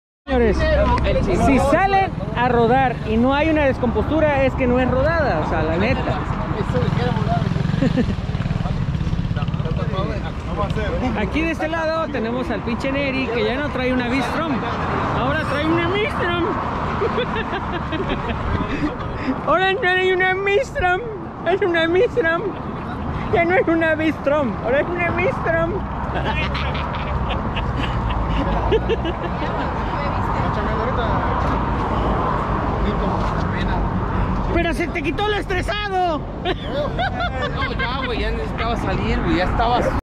Como cuando antes tenías una Vstrom650 y ahora tienes una ñistrom 😂
Un video algo diferente a lo que normalmente subo banda , un poco de humor. Esto fue parte de una típica rodada , en la cual siempre hay alguna moto que se descompone en el camino , pero es parte de la aventura amigos, Buenas Rutas y Gracias por el apoyo.